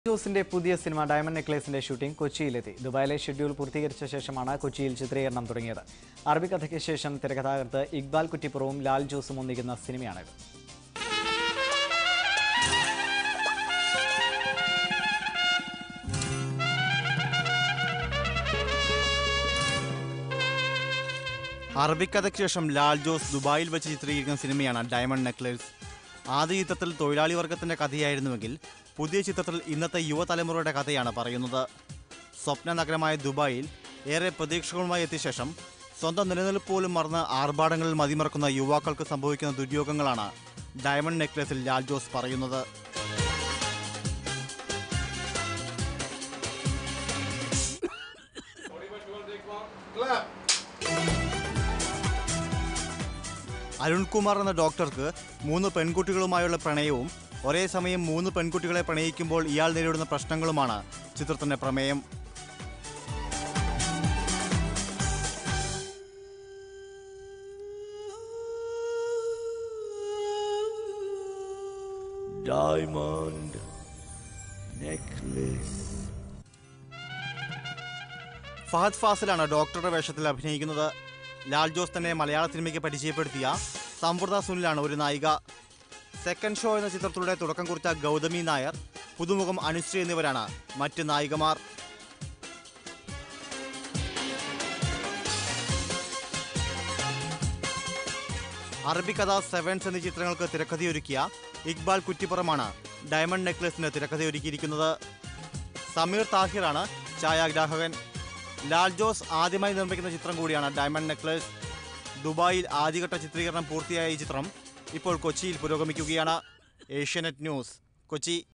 Diamond Necklace Shooting Location on Kochi
Lal Jose new Movie " Diamond Necklace"2nd SheduleShooting Location Started on Kochi